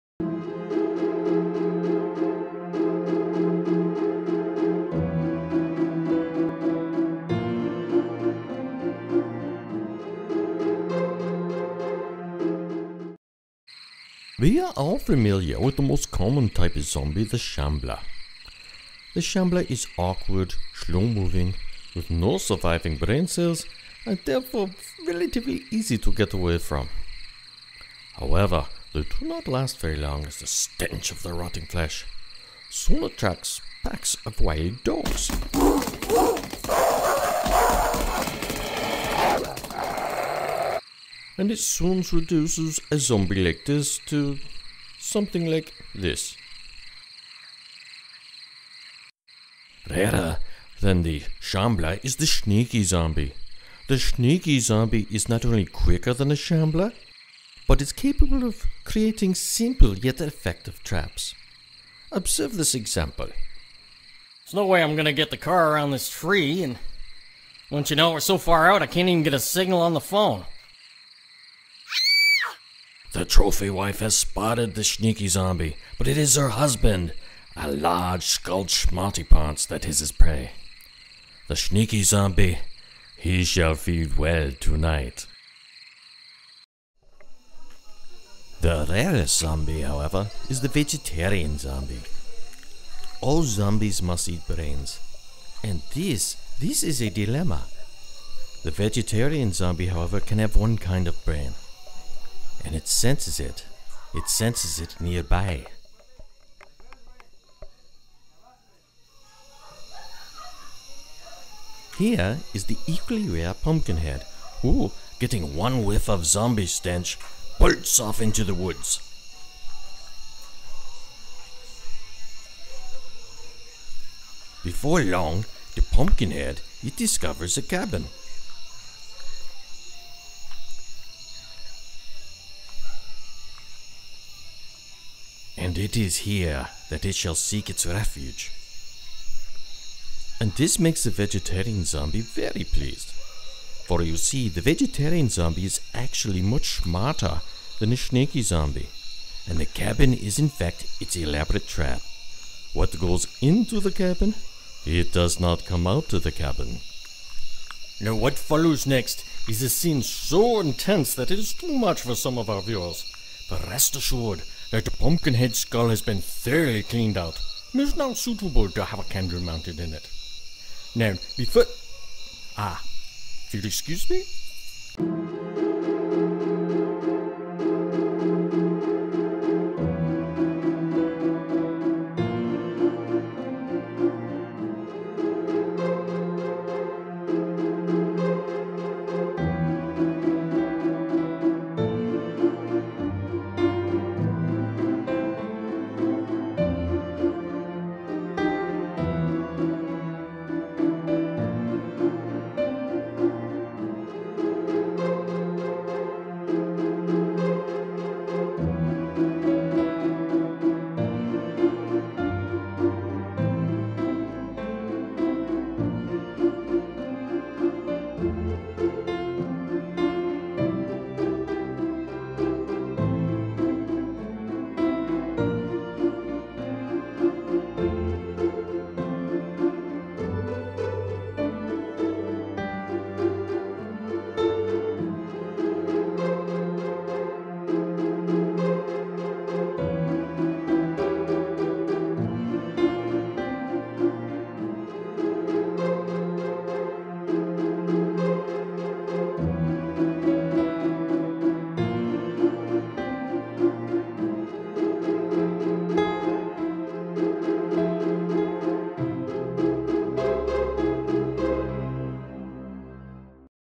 The Rarest Kind Of Zombie
A short documentary explaining what the rarest kind of Zombie is.